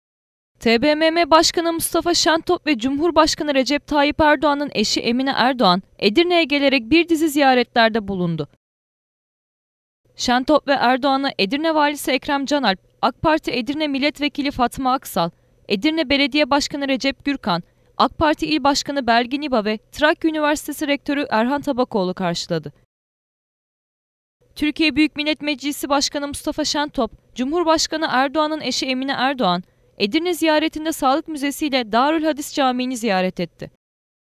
TBMM BAŞKANI ŞENTOP VE EMİNE ERDOĞAN’DAN EDİRNE ZİYARETİ
TBMM Başkanı Mustafa Şentop ile Cumhurbaşkanı Recep Tayyip Erdoğan’ın eşi Emine Erdoğan, Edirne’ye gelerek bir dizi ziyaretlerde bulundu.